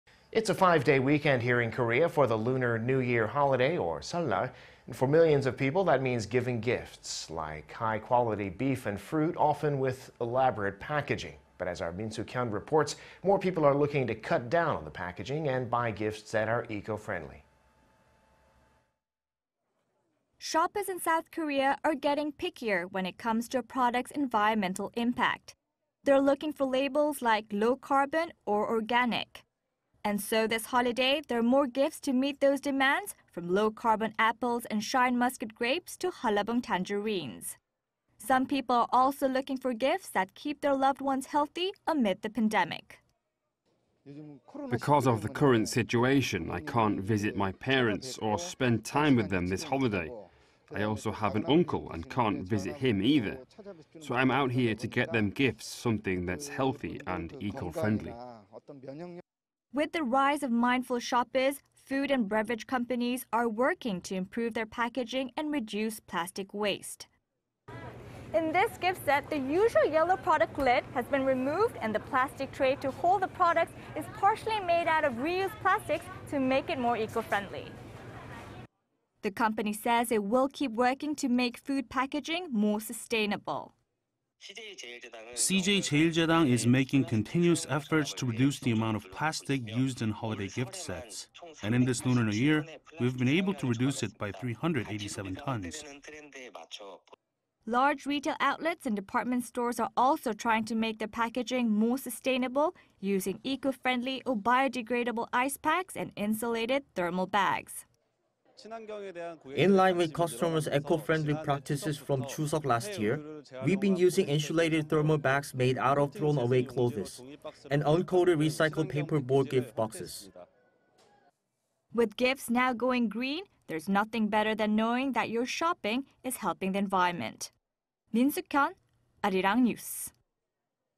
More S. Koreans shopping for eco-friendly gifts for this Lunar New Year
올해 설 "저탄소.유기농 선물세트 인기...포장도 친환경"

It's a five-day weekend here in Korea for the Lunar New Year holiday, or Seollal.
For millions of people, that means giving gifts like high-quality beef and fruit often with elaborate packaging.
But as our Min Suk-hyen reports, more people are looking to cut down on the packaging... and buy gifts that are eco-friendly.
Shoppers in South Korea are getting pickier... when it comes to a product's environmental impact.
They're looking for labels like "low-carbon" or "organic".
And so this holiday, there more gifts to meet those demands...from "low-carbon" apples and shine muscat grapes to Hallabong  tangerines.
Some people are also looking for gifts that keep their loved ones healthy amid the pandemic.

"Because of the current situation, I can't visit my parents or spend time with them this holiday. I also have an uncle and can't visit him either. So I'm out here to get them gifts, something that's healthy and eco-friendly."

With the rise of mindful shoppers, food and beverage companies are working to improve their packaging and reduce plastic waste.

"In this gift set, the usual yellow product lid has been removed and the plastic tray to hold the products is partially made out of re-used plastics to make it more eco-friendly. "

The company says it will keep working to make food packaging more sustainable.

"CJ CheilJedang is making continuous efforts to reduce the amount of plastic used in holiday gift sets. And in this Lunar New Year, we've been able to reduce it by 387 tons."

Large retail outlets and department stores are also trying to make their packaging more sustainable ... using eco-friendly or biodegradable ice-packs and insulated thermal bags.

"In line with customers' eco-friendly practices...from Chuseok last year, we've been using insulated thermal bags made out of thrown away clothes... and uncoated recycled paperboard gift boxes."

With gifts now going "green", there's nothing better than knowing that your shopping is helping the environment.
Min Suk-hyen, Arirang News.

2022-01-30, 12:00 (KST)